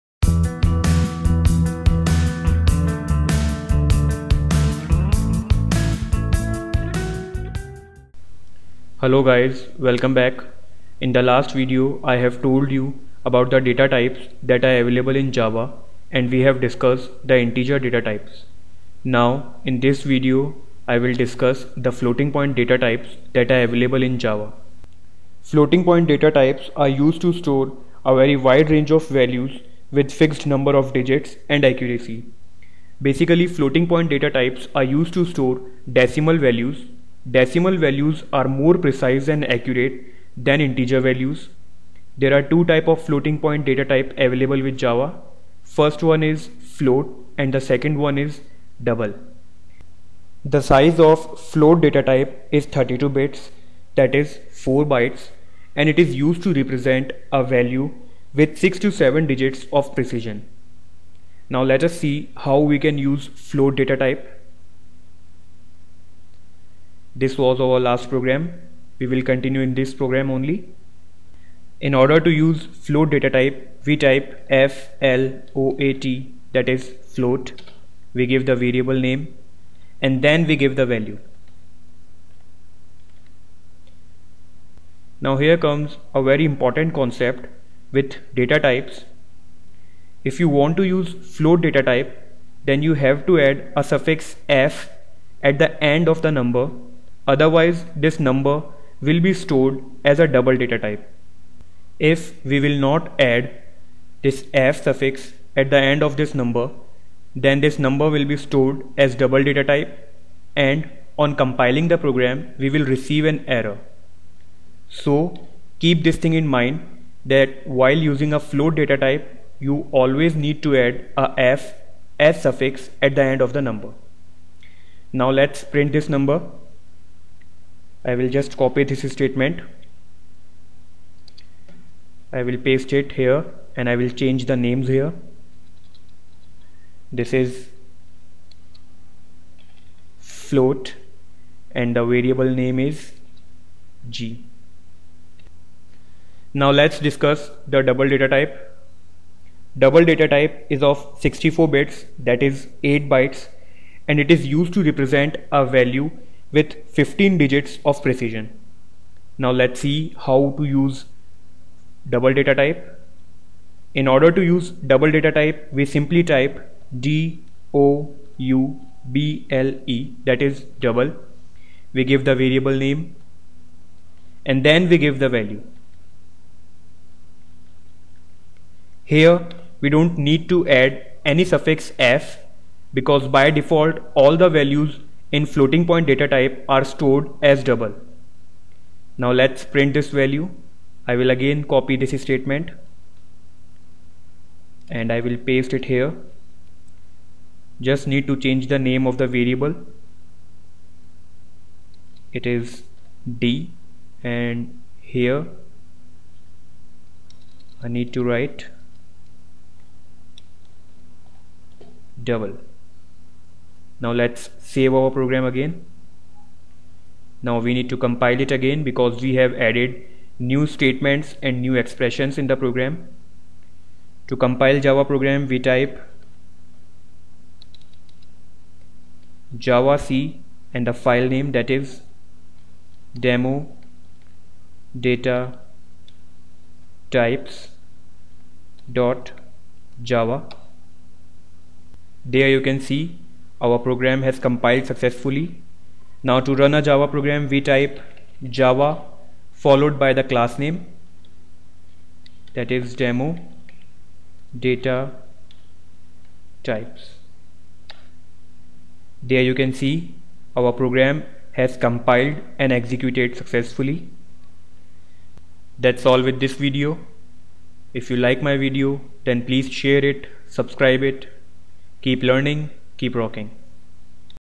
Java Tutorials Floating Point Data Type
In this video you will get learn about the second categorization of  java data types. The second type of data types that are  available with java is Floating Point Data Type. Floating Point Data type is used to represent wide variety of values.Basically floating point data type is used to store decimal values. Decimal values are more accurate and precise then integer values.Now floating point data types are used to store values with decimal values.Floating point data type contains two types of data type

1. Float
2. Double

Float data type is used to store decimal values with 6 to 7 digits of precession .It is of 32 bits i.e. 4 bytes. By default all floating values or decimal values are stored in double type .So i you want to use float data type then you have to add a suffix "f " at the end of the number otherwise it will store the value to double type and you have given data type float so an error will come.

Double data type is used to represent values with precession of about 15 digits. Its size is 64  i.e. 8 bytes. Double data type is used to store very big values and that big value is not used so often so we use float data type then double data type.